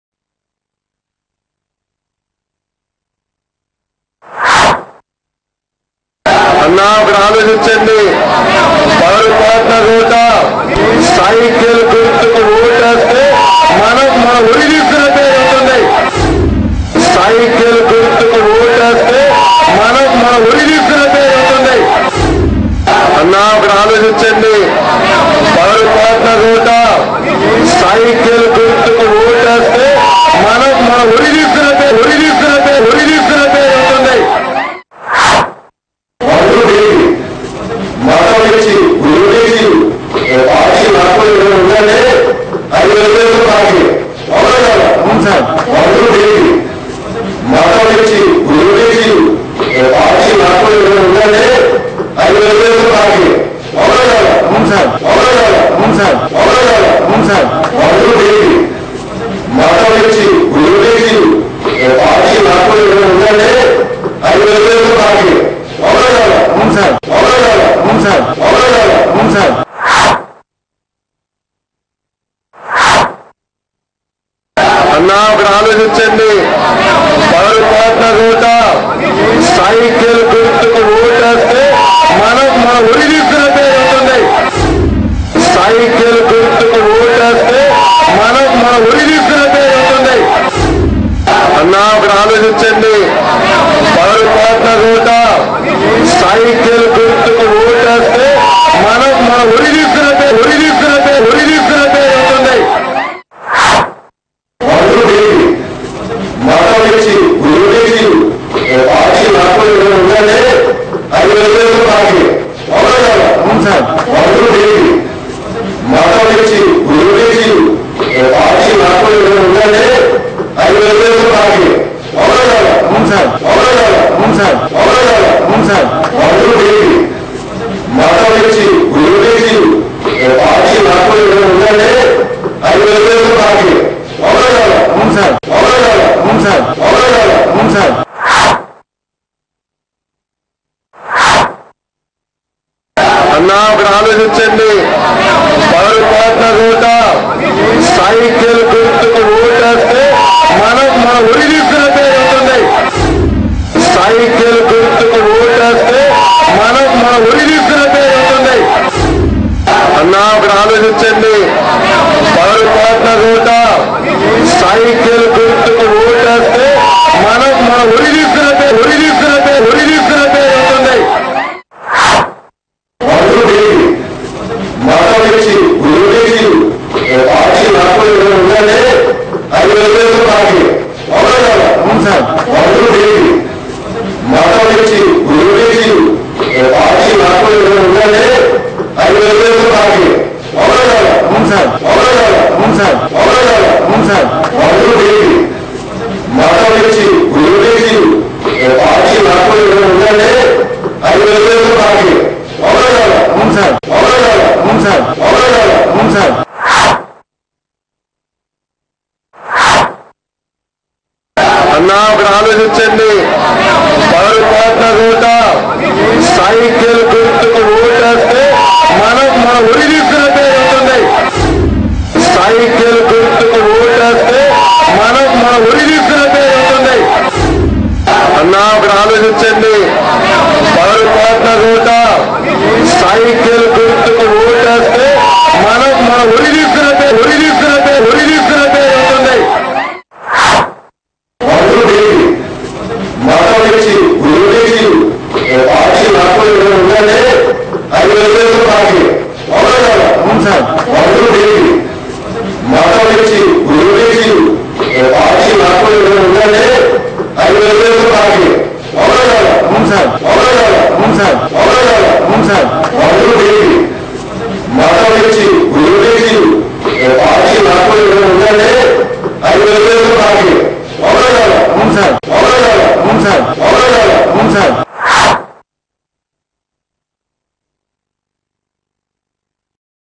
NARA LOKESH LATEST SPEECH AGINST TO HIS FATHER AND AP CM CHANDRABABU NAIDU
NARA LOKESH MANASULO MATA BAYATAPADINDHI CHUDANDI IN ELECTION MOMENT AGAINST TO HIS FATHER

LATEST NEWS FROM TDP  NARA LOKESH AGAINST TO AP CM CHANDRABABU NAIDU